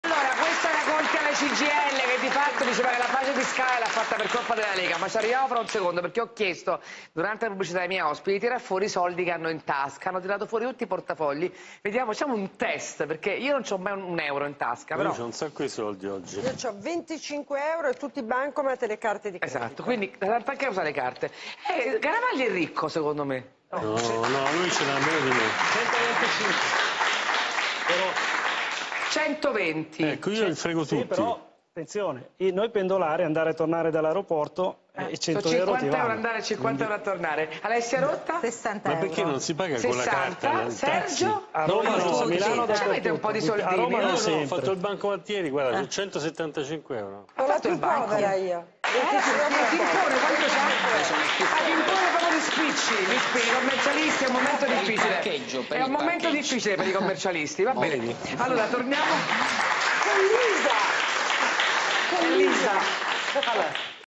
Massimo Garavaglia: "Pagare il taxi con la carta bancomat? A Roma non sempre, a Milano dappertutto"
Myrta Merlino fa un piccolo test per vedere quanti soldi in contanti abbiano gli ospiti in studio nel proprio portafogli: Daniela Santanchè 25 euro, Sergio Rizzo 175 euro, il commercialista Gianluca Timpone pochi spicci.